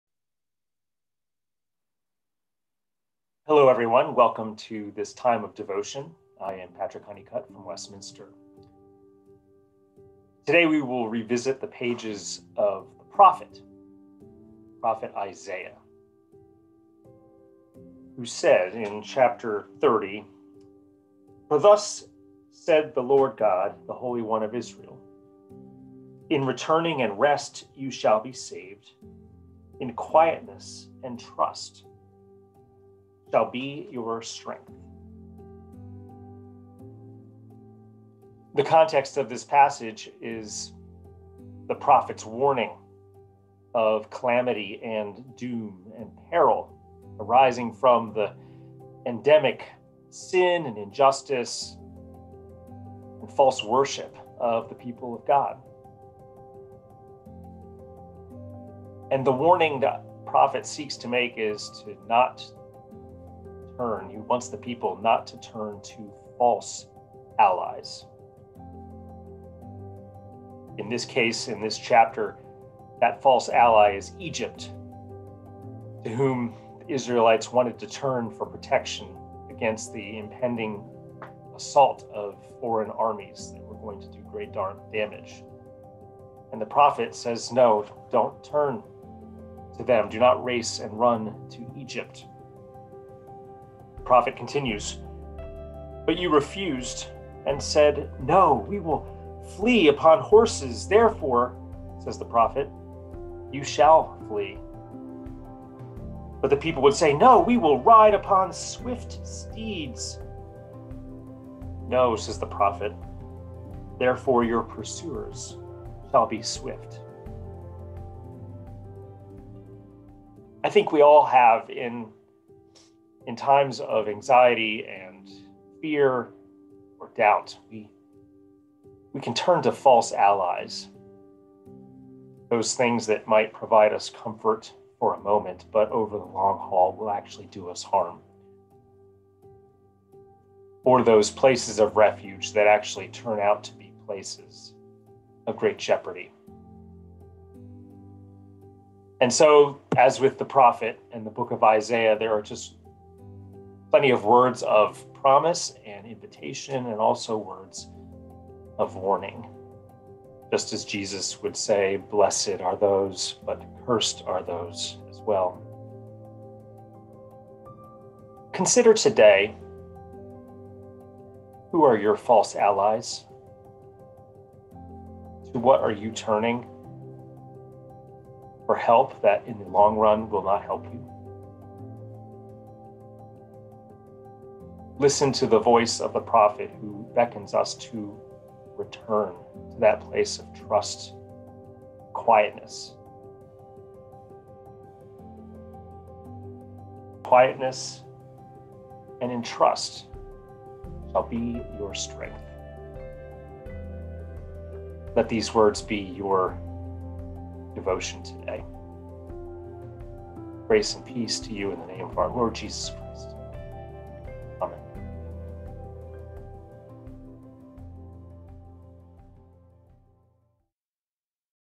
WPC Daily Devotional - March 4, 2021
I think we all have—in times of anxiety, and fear, and doubt—turned to false allies: those things that might provide us comfort for a moment, but over the long-haul will actually do us harm.

Daily Devotional from Westminster Presbyterian Church in Alexandria, VA. Westminster is a worshipping community deeply committed to study, service, and sharing God’s love.